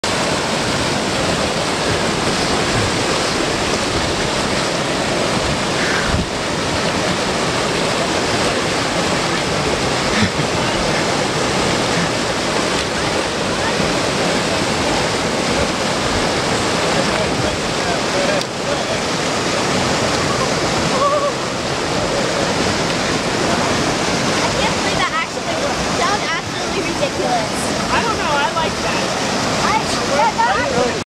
PCT Thruhike fording Matterhorn Creek
2011 PCT Thruhike fording Matterhorn Canyon Creek.   Balls, Sunshine, Bottlerocket, Wired, Snowblind, and Scouts Honor all crossing at once.Our Blog is